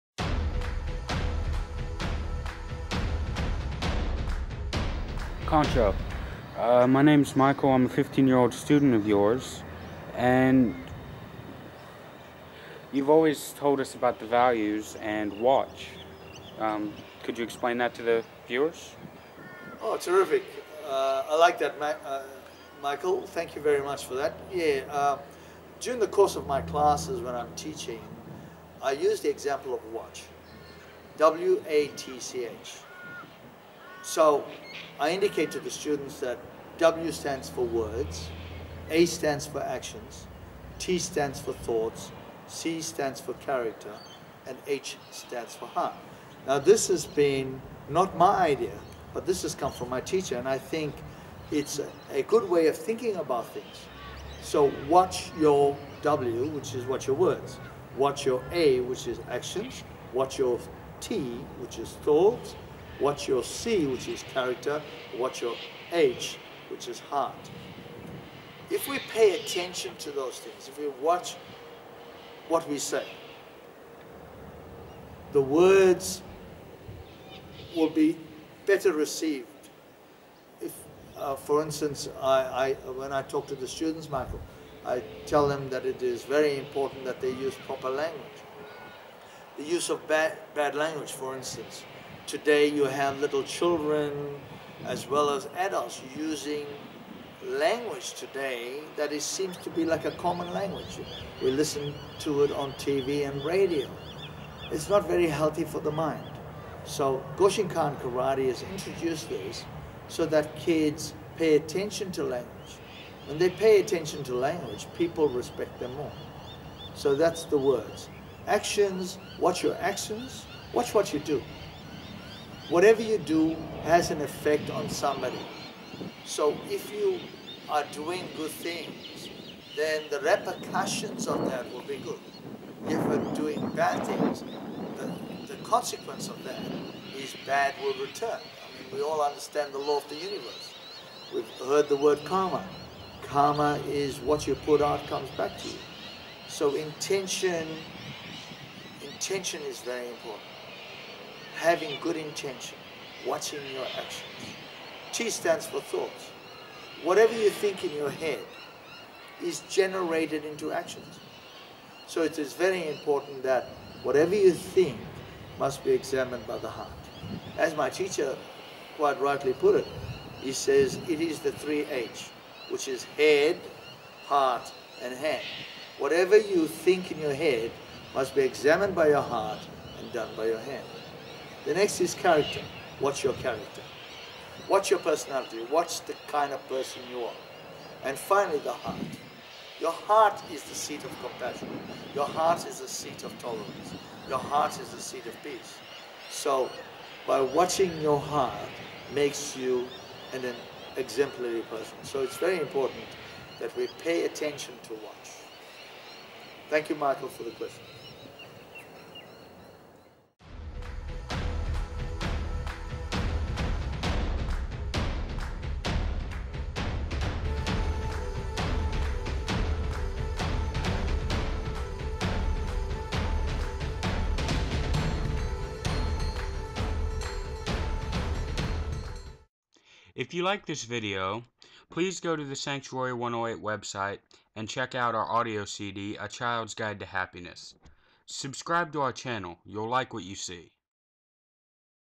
What is WATCH?
In this clip Kancho explains the values associated with the word WATCH.  The interview is done by one of our younger students, he is also the editor for the clip.